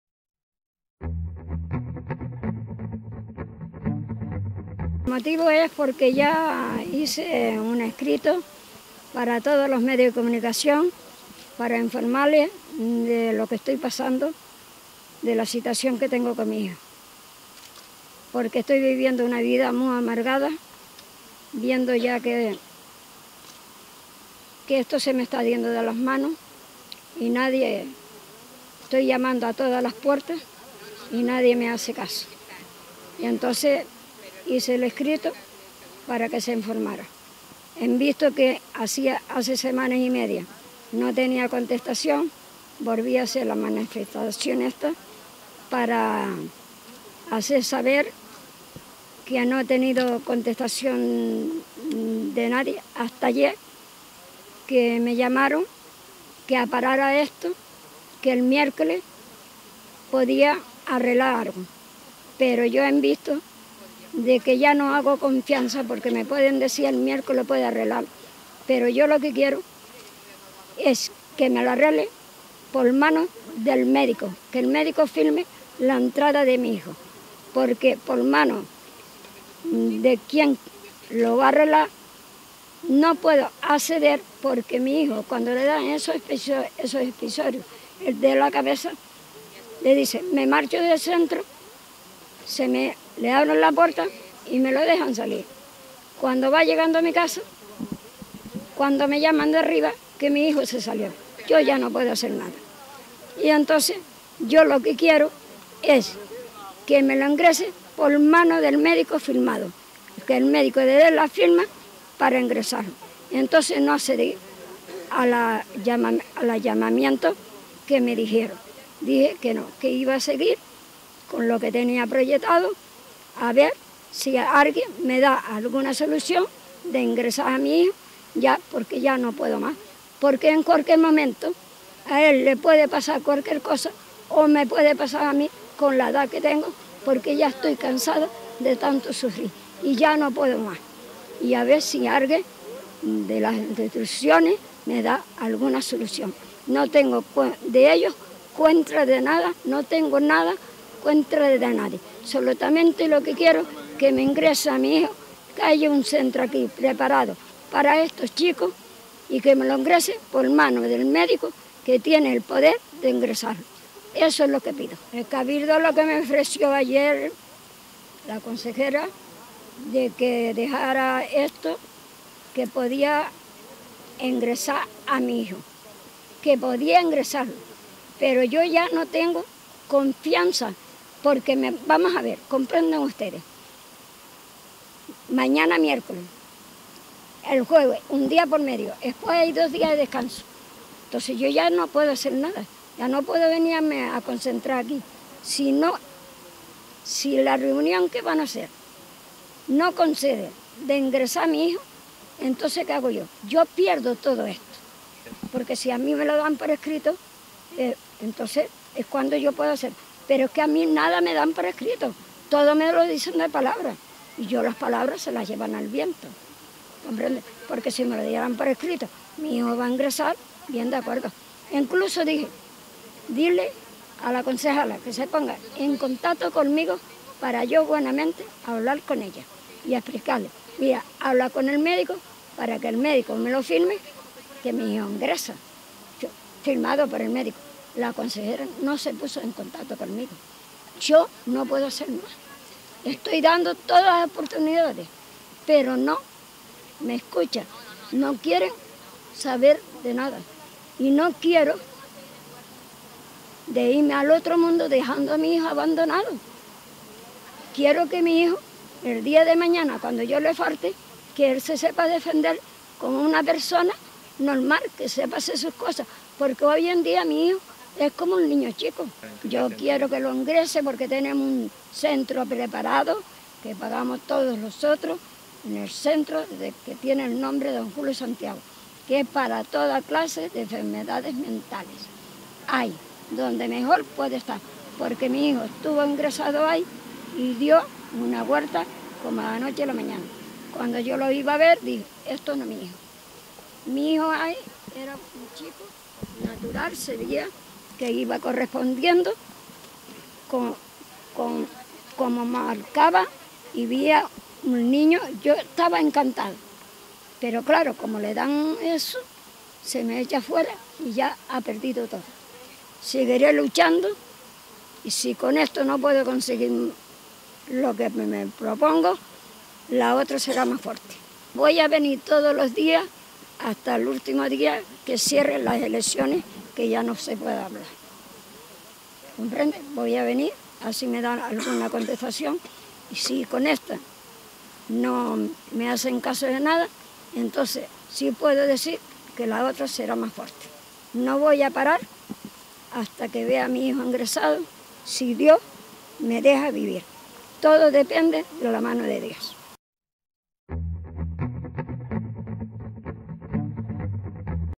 Arrocha protesta a las puertas del Cabildo: “No voy a parar hasta que vea a mi hijo ingresado”
María Isabel Arrocha, de 73 años, llevó este martes a las puertas del Cabildo de Lanzarote su reivindicación de que se ingrese a su hijo en la Unidad de rehabilitación psicosocial Julio Santiago Obeso. “No voy a parar hasta que vea a mi hijo ingresado”, afirma.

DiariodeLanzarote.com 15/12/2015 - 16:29

Su hijo, de 39 años de edad, tiene un diagnóstico de “retraso mental leve, trastorno histriónico de la personalidad y politoxicología” y, de hecho, ya estuvo ingresado en el citado centro meses atrás.

Arrocha reclama a las instituciones públicas que se impliquen para buscar una solución a la situación de su hijo y que se decrete su ingreso por orden médica, para evitar que “se marche del centro” cuando le dan “episodios” de crisis.

La madre asegura que la situación se está “yendo de las manos”, que ha “llamado a todas las puertas” y que nadie le ha hecho “caso”.

El lunes recibió una llamada de Servicios Sociales del Cabildo para que “parara” las acciones de protesta, que ha comenzado este martes y tiene pensado continuar hasta las elecciones del domingo.

María Isabel Arrocha dice que no tiene “confianza” en las instituciones públicas y reclama atención para su hijo, a quien “le puede pasar cualquier cosa”. “No quiero irme al otro mundo dejando a mi hijo abandonado, sin que se sepa defender como una persona normal”, afirma.

Arrocha explica que cuando su hijo estuvo ingresado en el centro Julio Santiago su estado “dio una vuelta”, como “de la noche a la mañana”. “Ahí es donde mejor puede estar”, señala.